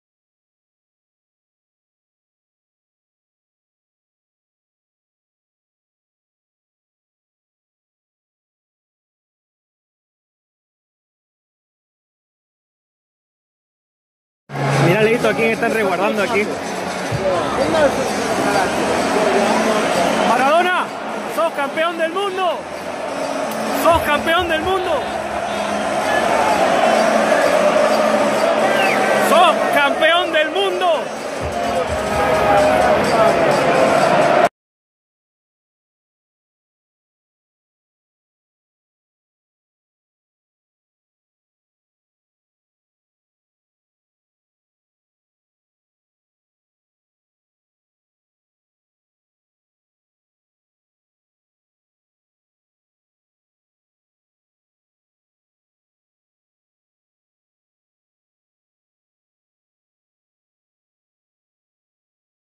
Maradona middle finger reaction on Rojo Goal vs Nigeria and all emotions - Argentina 2 - 1 Nigeria
Maradona middle finger reaction on Marcos Rojo Goal vs Nigeria - Argentina 2 - 1 Nigeria - World cup. New Video from stands
Messi goal
Rojo Goal
Mikel Goal